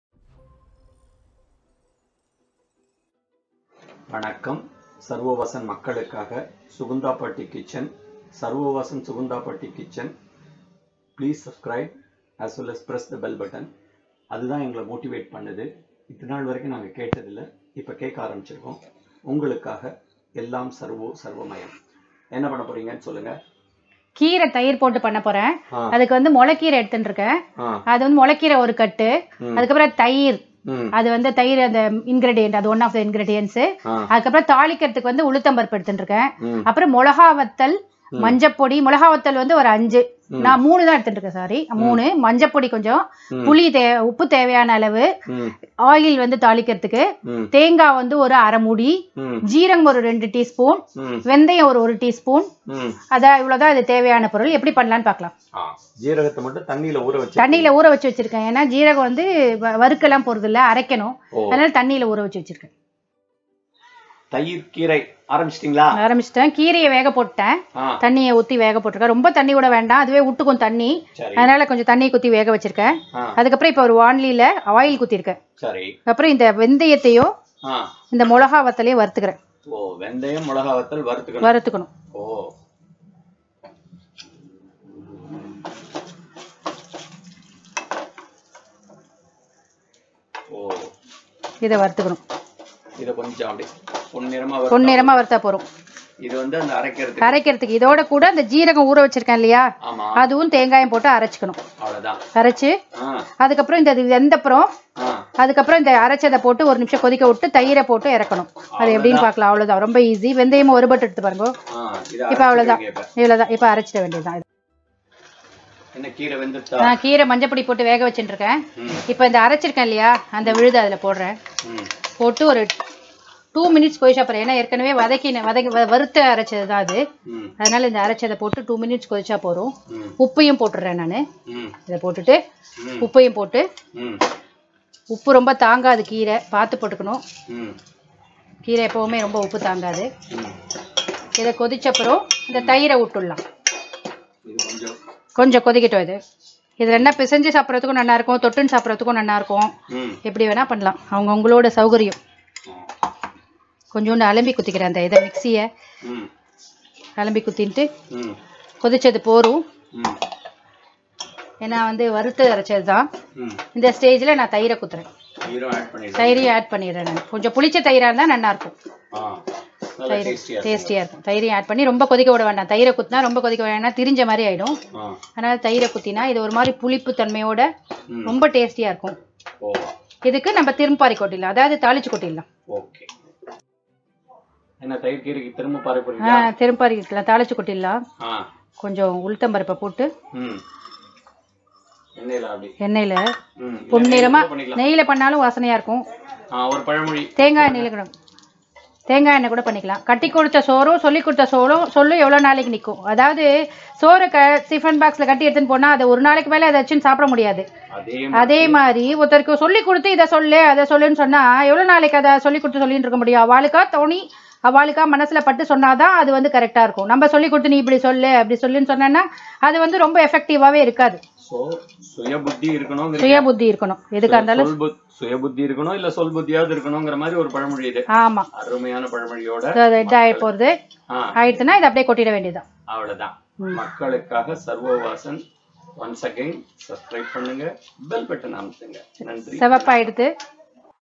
தயிர் கீரைCURD GREENS GRAVY
# தயிர் விட்ட கீரை மசியல் பண்ணி பாருங்க
# தயிர் கீரை,# curd green  gravy# முள கீரை தயிர்,மிளகாய் வற்றல் ,மஞ்ச பொடி, உப்பு, துருவிய தேங்காய், எண்ணை,ஜீரகம் உளுதம் பருப்பு